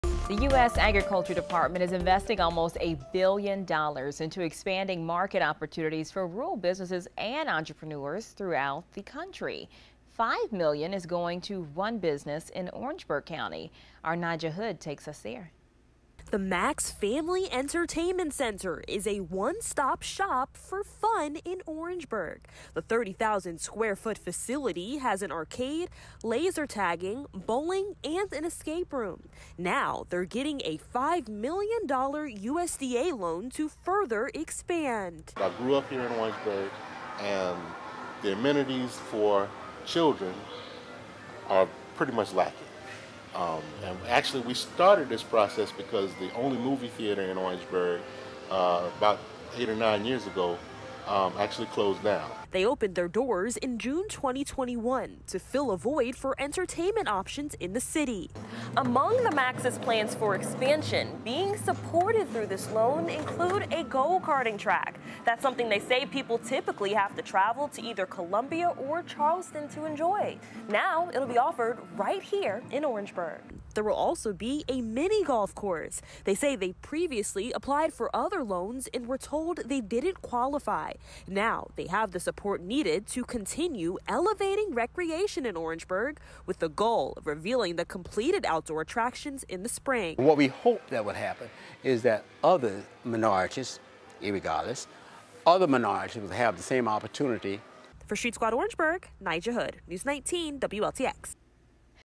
Orangeburg business expanding recreation opportunities with the help of $5 million federal loan
Among The Max Family Entertainment Center's plans for expansion include a go karting track and miniature golfing course. More